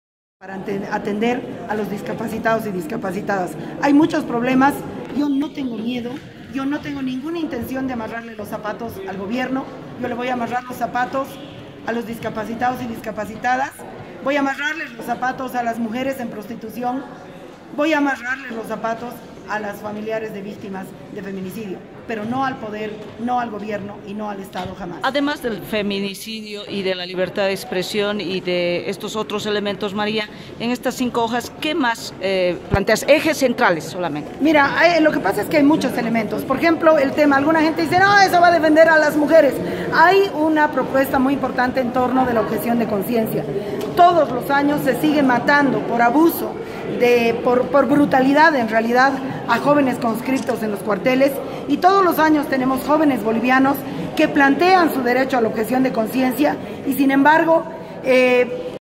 La feminista María Galindo se inscribió para el Defensor del Pueblo
Galindo llevó encima alusiones simbólicas por la defensa de los derechos humanos de las mujeres y pueblos indígenas.